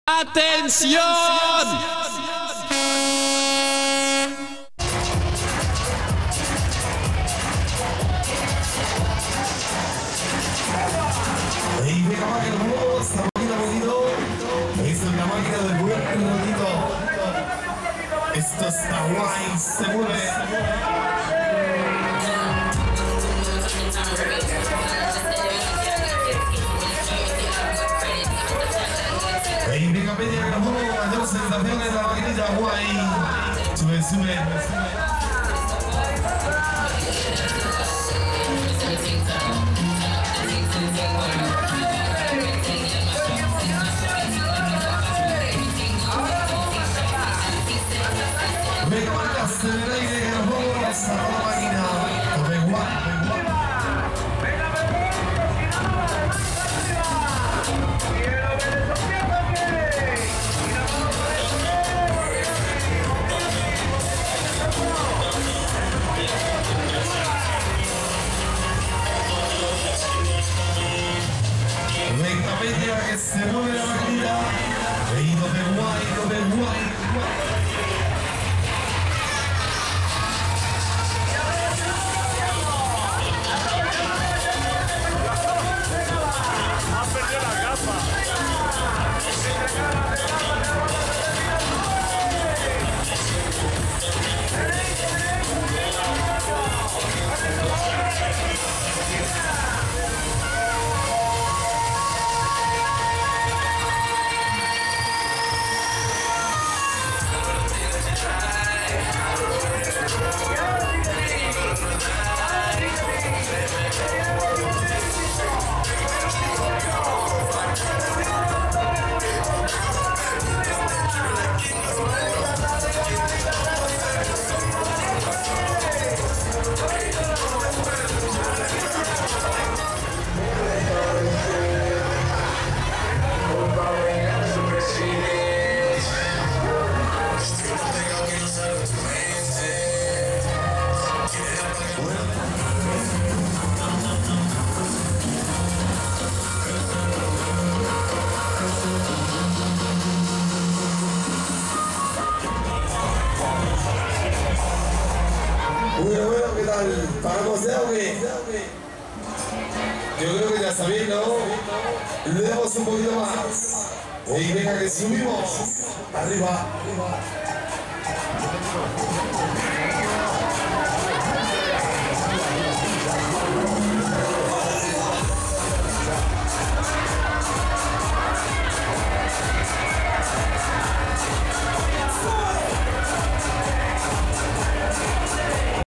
Move It (K.M.G) Feria de San Isidro de Talavera de la Reina 2.018
Move It (K.M.G) Atracciones Silva en la Feria de San Iisdro 2.018 de Talavera de la Reina.